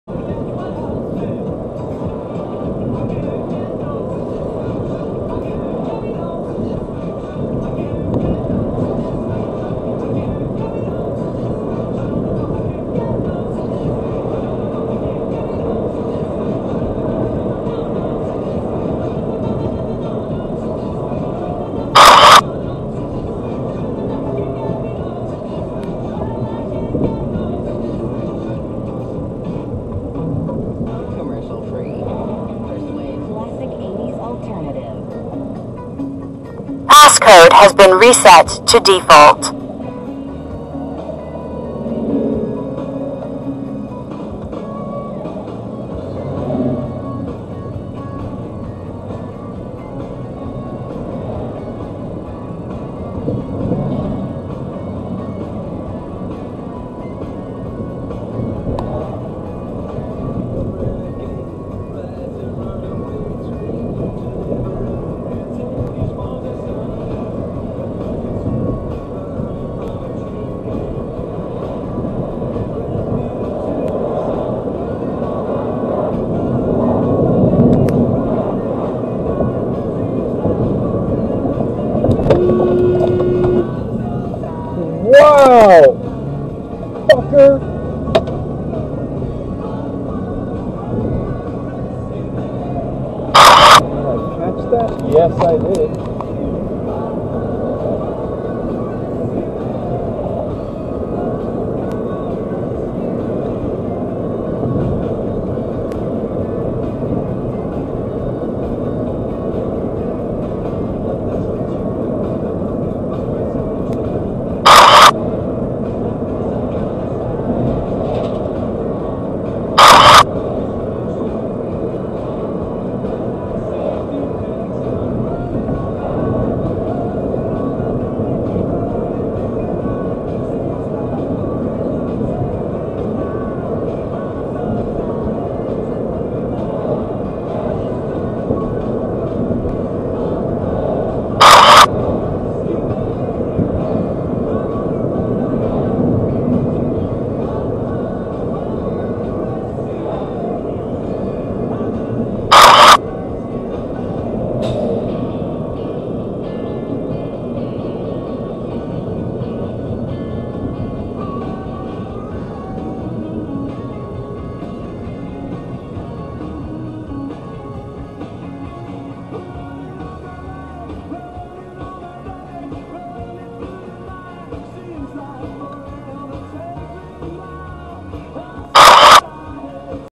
Hyperion Tempe
Ungrateful driver merging to the road with thinking.